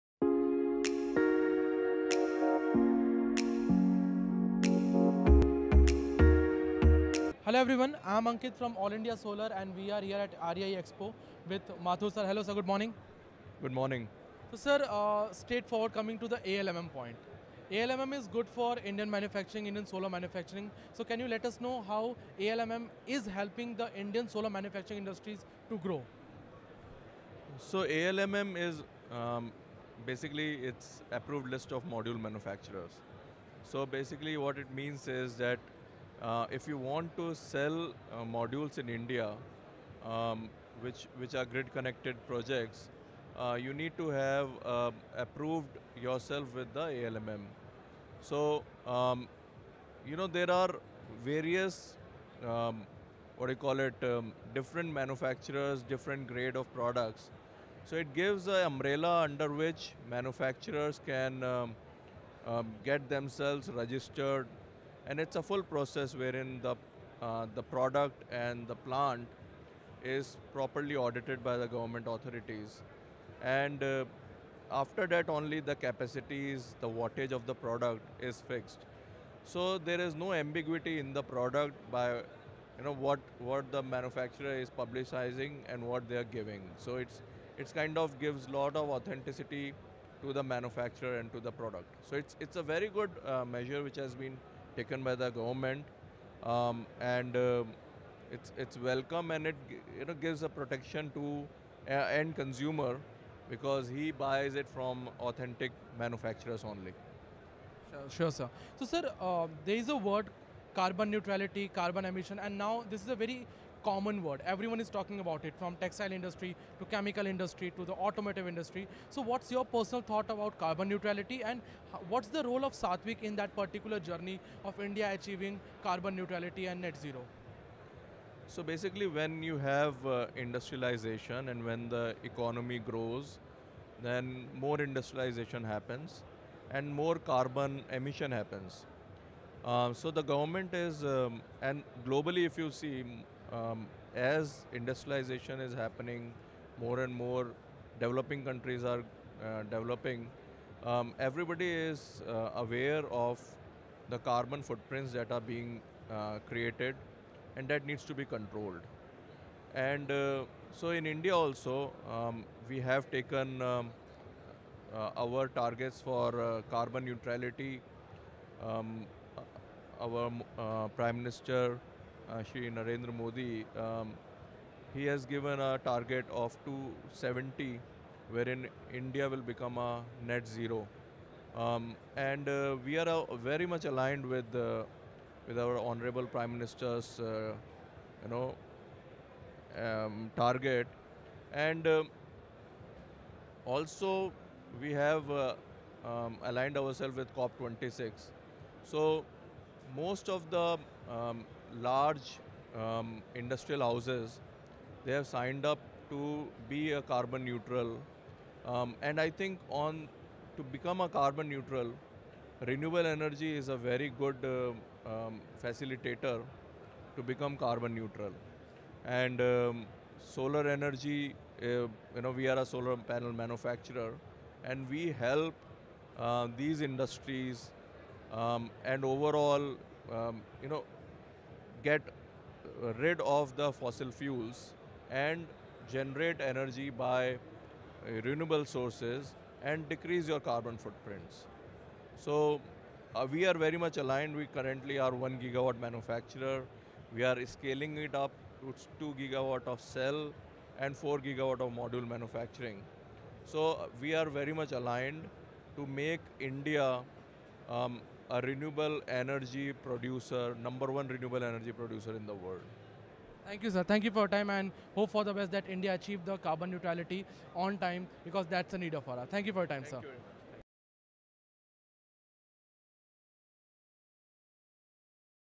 Prashant Mathur from Satvik Green | What Solar Industry Thinks about Net Zero | All India Solar
In this video, Mr. Prashant Mathur, who is the CEO of Satvik Green Energy  shared his views on the Global Target for Net Zero.

Through All India Solar, our objective is to make people aware about the Net Zero by educating them about Solar Energy and Energy Efficiency.